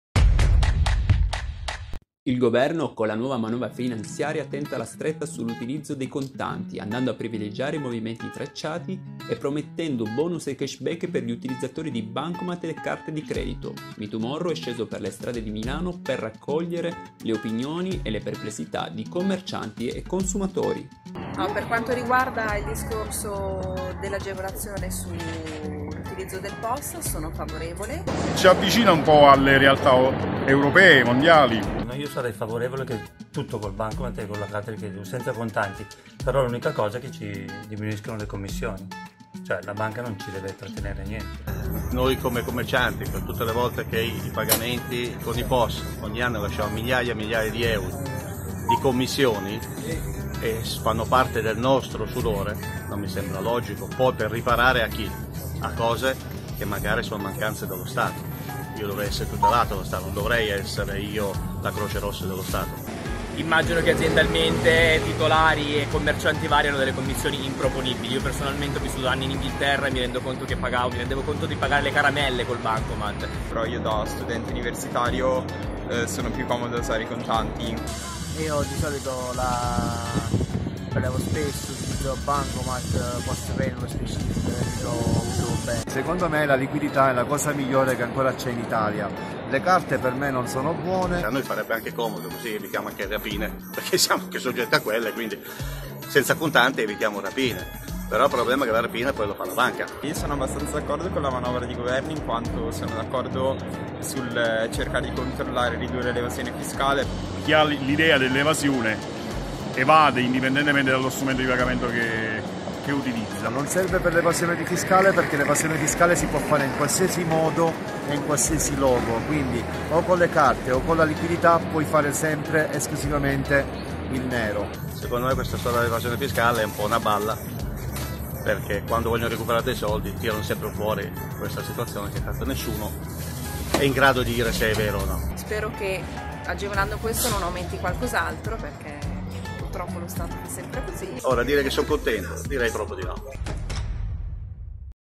Finanziaria: stretta sul contante, i milanesi rispondono
07.10.19 • Dal Governo nuove incentivazioni all’utilizzo della moneta elettronica: giro di opinioni tra commercianti e cittadini milanesi, tutti pressoché concordi sul nodo commissioni.
🎤 Fabio Implicito